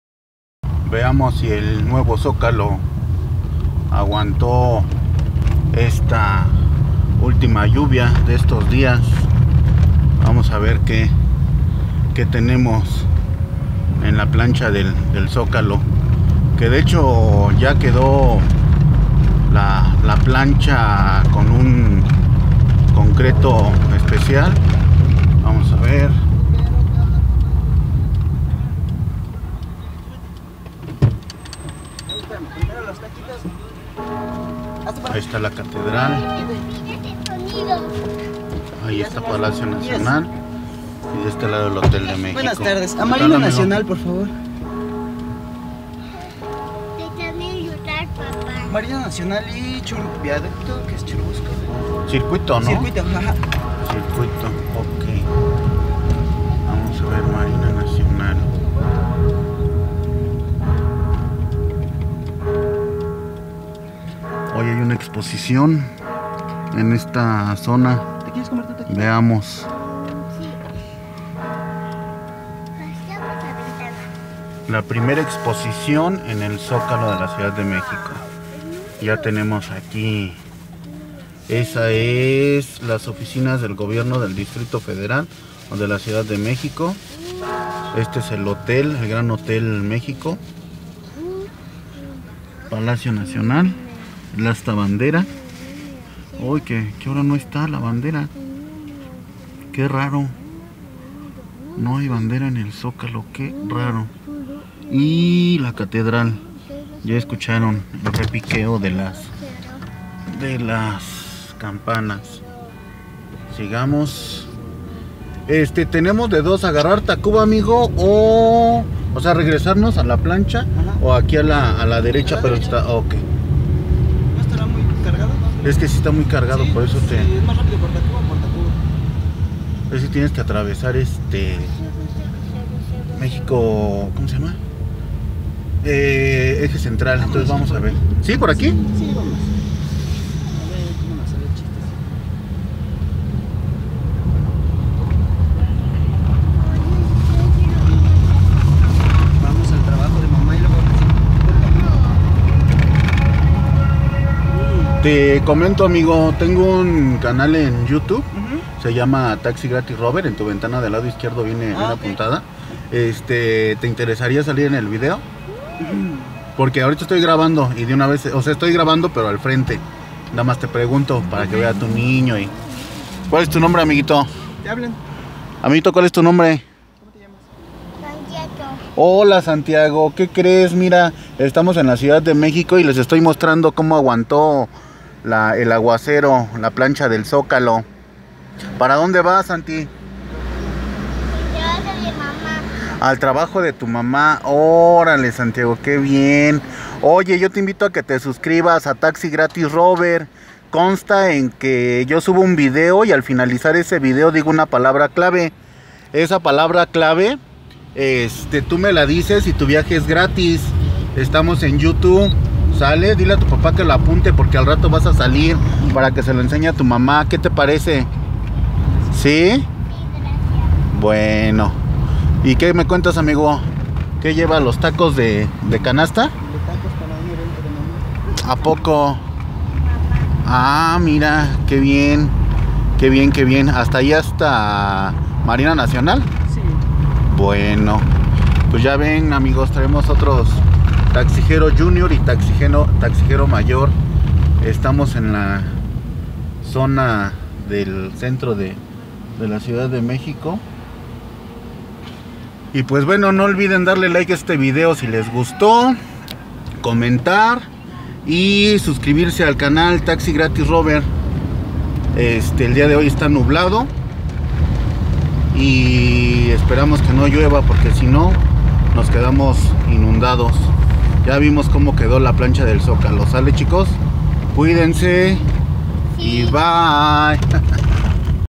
EL NUEVO ZOCALO DE LA CIUDAD DE MEXICO Y SU PRIMER EXPOSICIÓN DE CULTURAS INDIGENA
PRESENTAMOS LA PRIMERA EXPOSICIÓN EN EL ZOCALO DESPUES QUE SE REHABILITO CON NUEVO CONCRETO, LUCES Y LAMPARAS, ADEMAS NOS ABORDA UN NIÑO CON SU PAPÀ QUE VAN EN CAMINO A MARINA NACIONAL CON SU MAMÀ, UNA TARDE NUBLADA